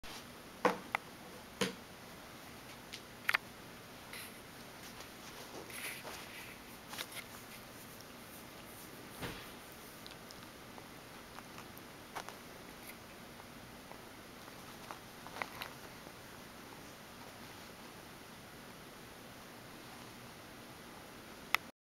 Kitten vs Camera Strap
Ebi, a two-month old grey tabby plays with a camera strap on her first day home.